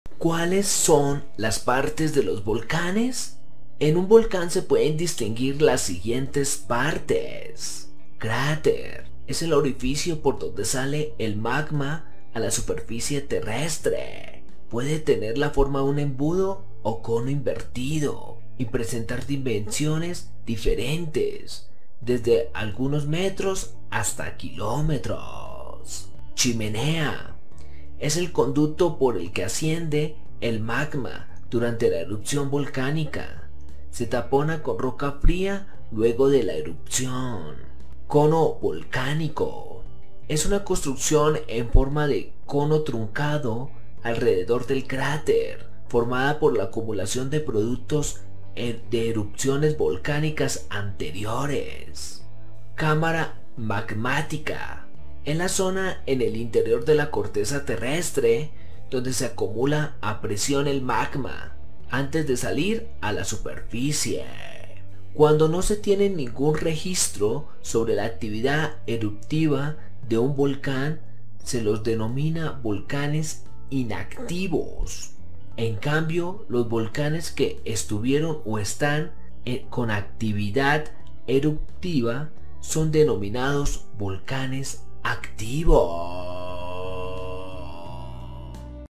# Metarquía / ¿Cuáles son las partes de los volcanes?
En este vídeo explicaré acerca de cuáles son las partes de los volvanes eso va ayudar a sobrellevar lo que vivimos, aquello que tocan los extremos de las emociones y los sentimientos.